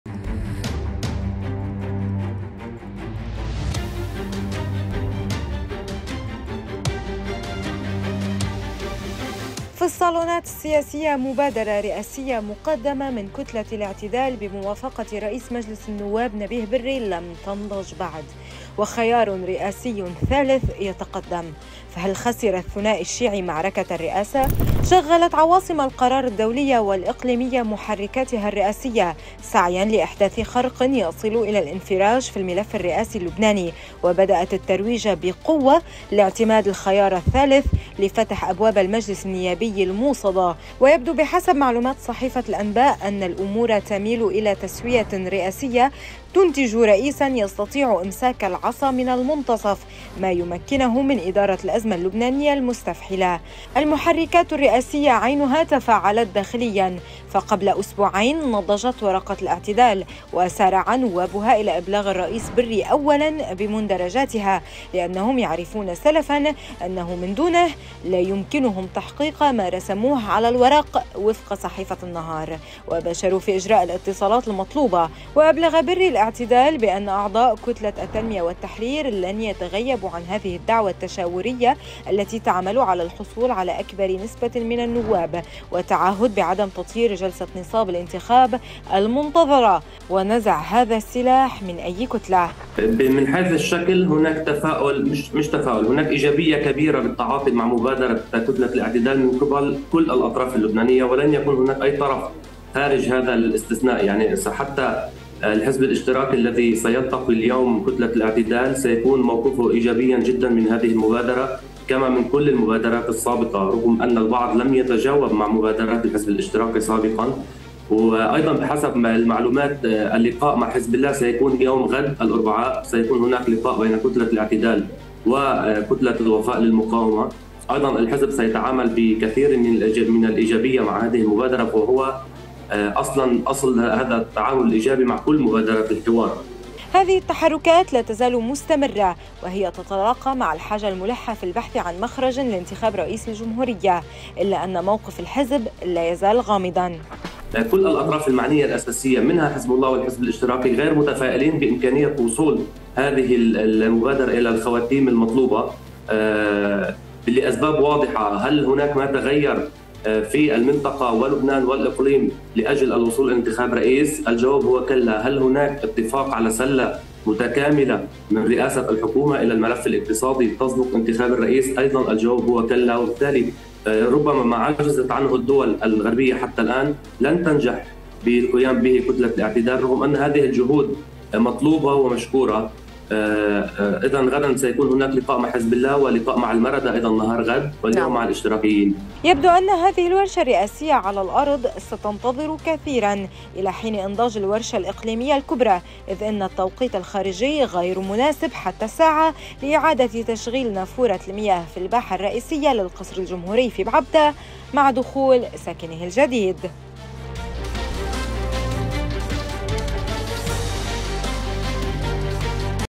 "خيار ثالث" للرئاسة يتقدم بقرار دولي .. والجديد تكشف: هل خسر الثنائي المعركة؟
في الصالونات السياسية مبادرة رئاسية مقدمة من كتلة الاعتدال بموافقة رئيس مجلس النواب نبيه بري لم تنضج بعد .. وخيار رئاسي ثالث يتقدم.. هل خسر الثنائي الشيعي معركة الرئاسة ؟